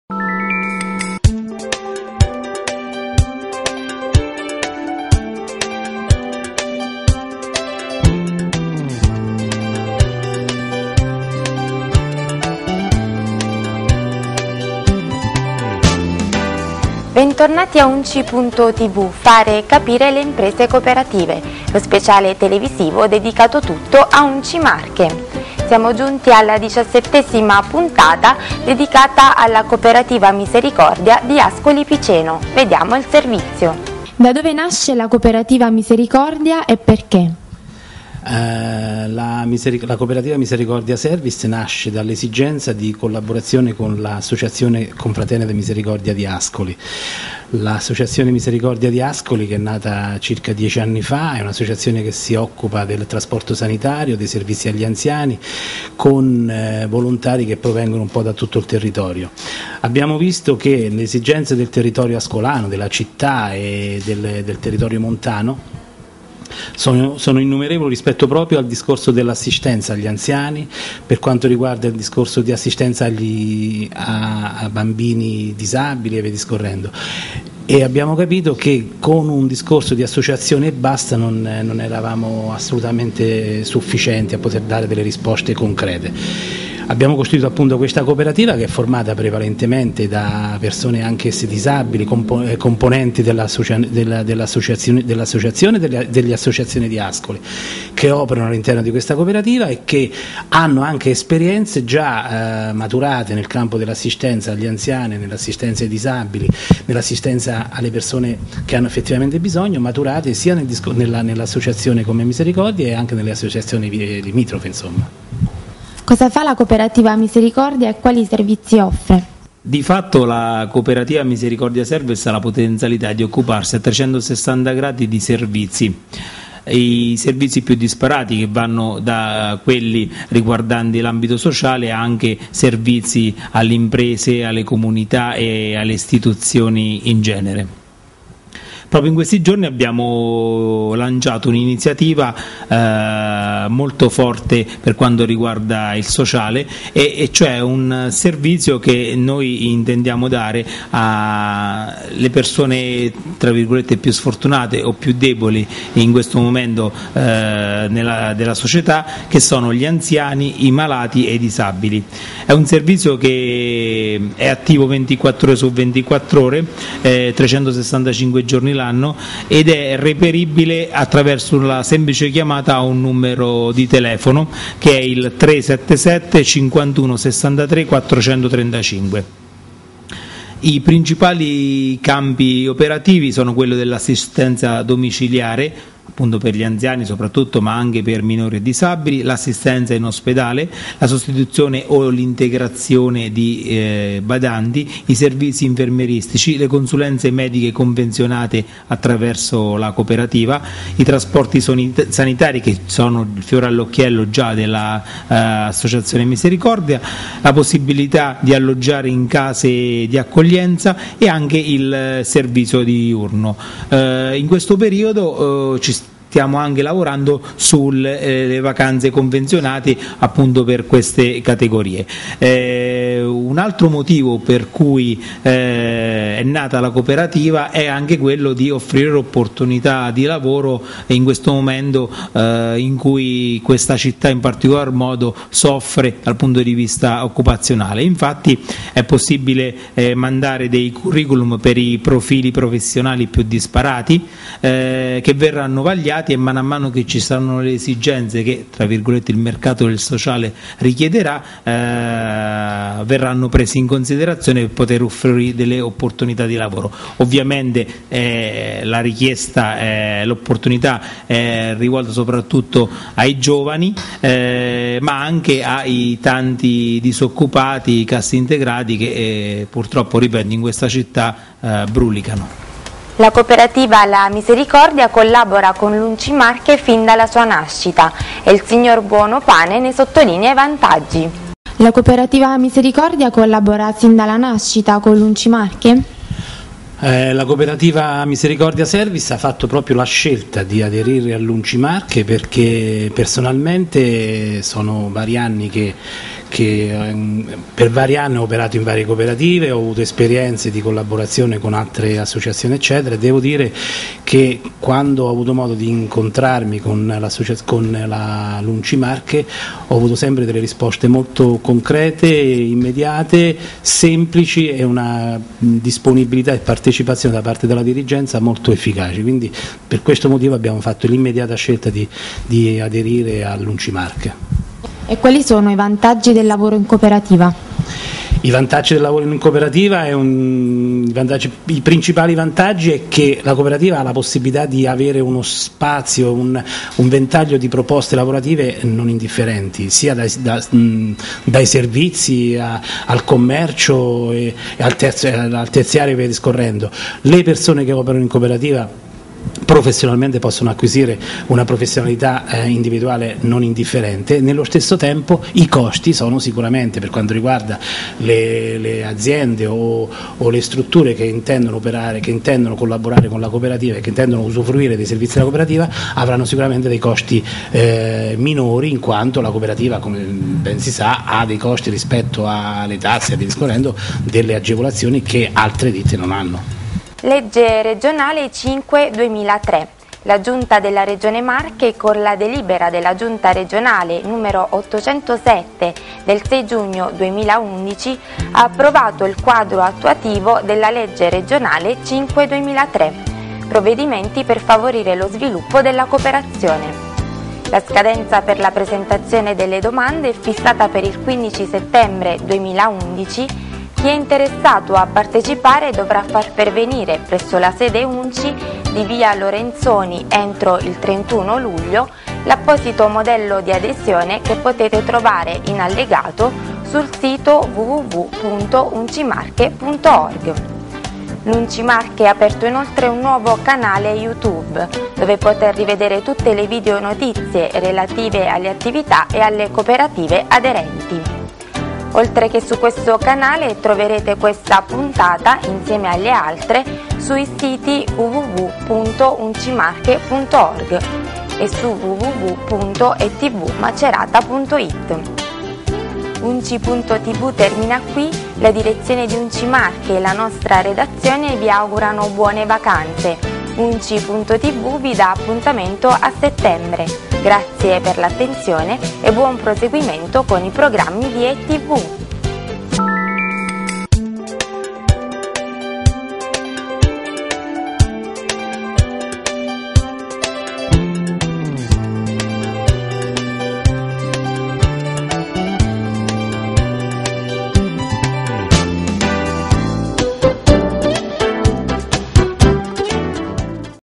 format UNCI.TV - COOPERATIVA MISERICORDIA
Magazine Ufficiale dell'Unci, Unione Nazionale Cooperative Italiane. Puntata 17.
Con le nostre telecamere siamo andati a conoscere la Cooperativa Misericordia di Ascoli Piceno.